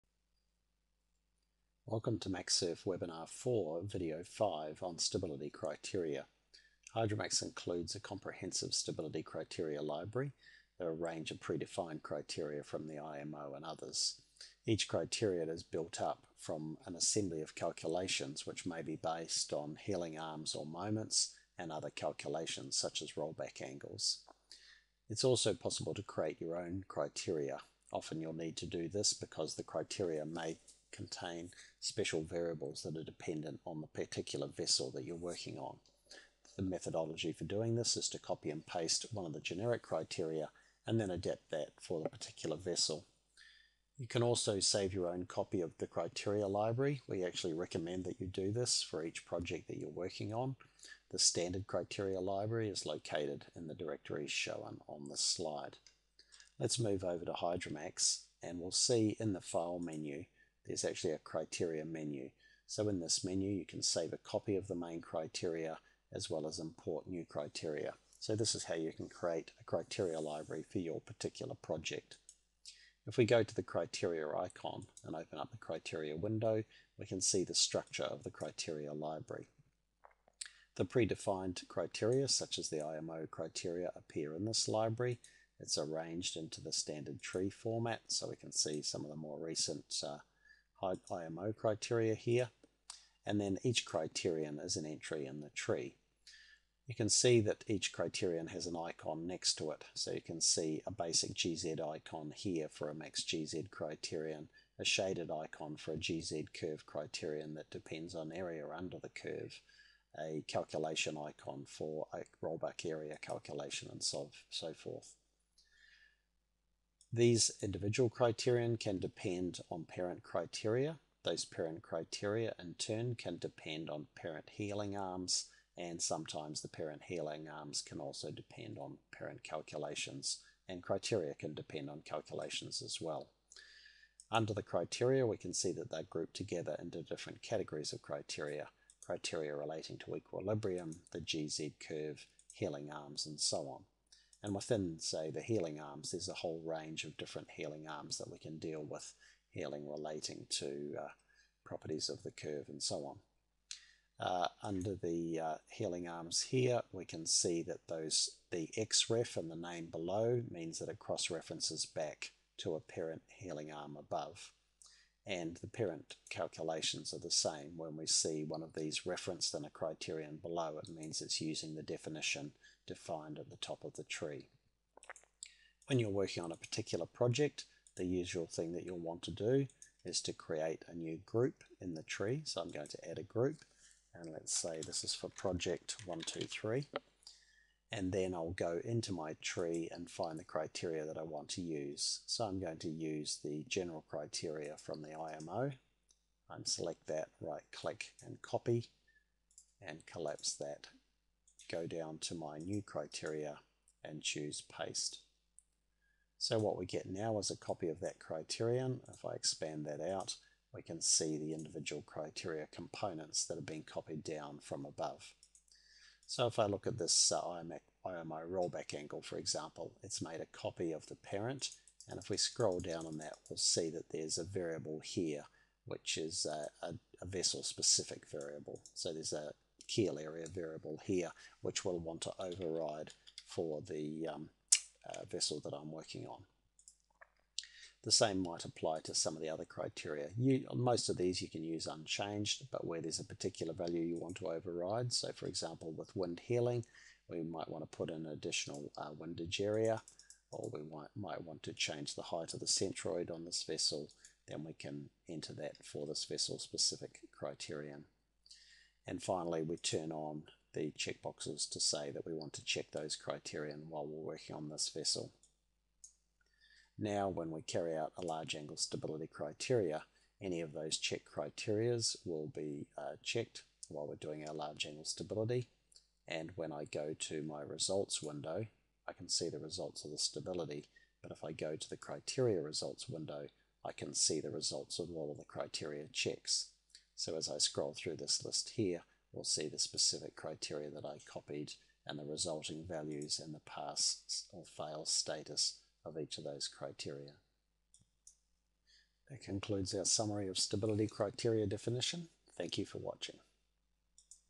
Stability Criteria
This video explains how to customise the standard stability criteria provided with Hydromax to define your own criteria library and criteria for a particular vessel.